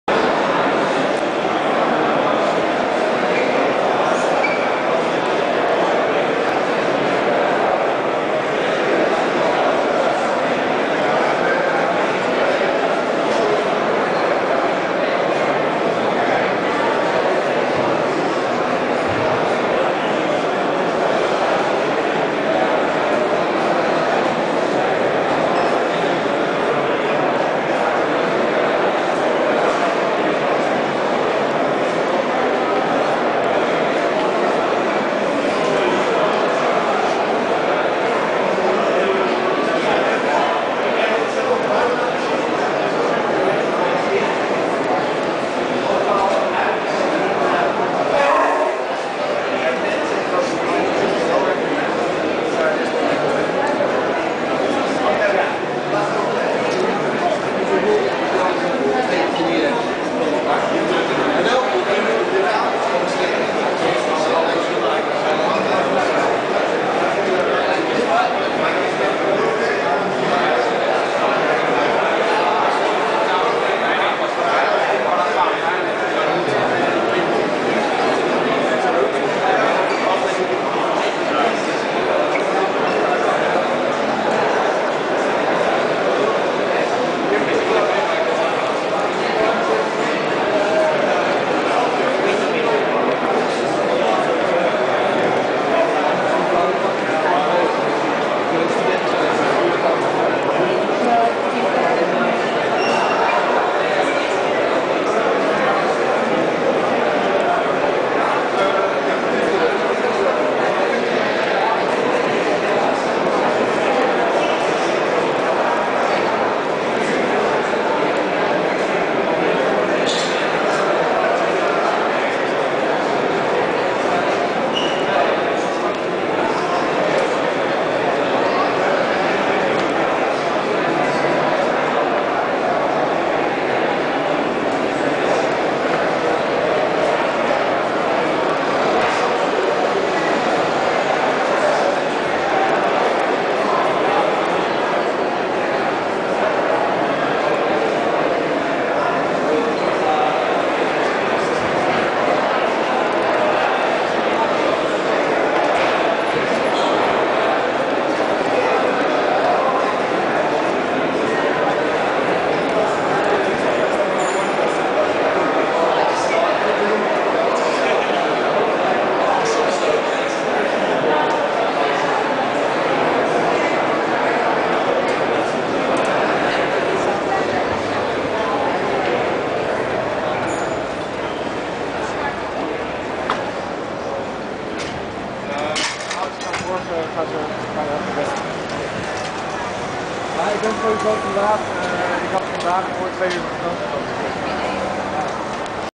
The Apple Store (Amsterdam March 13, 2012)
I visited the Apple Store in Amsterdam, Holland.  I had heard that it was "museum-like": it's quite true.